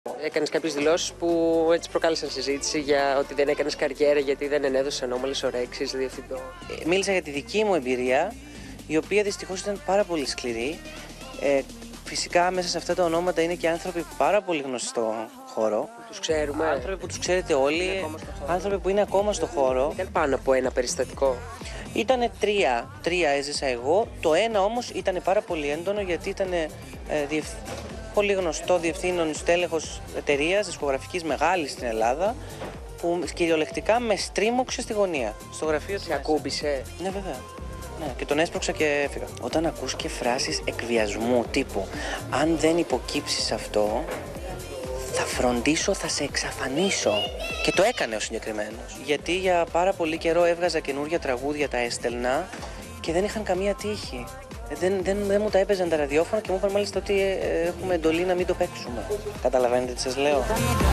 Ο Μαυρίκιος Μαυρικίου για κακοποιητικές συμπεριφορές και εκβιασμούς που βίωσε 18/11/2022 | OPEN TV
Γίνετε μέλος στο κανάλι του OPEN για να αποκτήσετε πρόσβαση σε προνόμια:
Μαυρίκιος Μαυρικίου: Περιγράφει ένα περιστατικό με γνωστό άνθρωπο που τον  παρενόχλησε και στη συνέχεια τον απείλησε ότι θα τον «εξαφανίσει».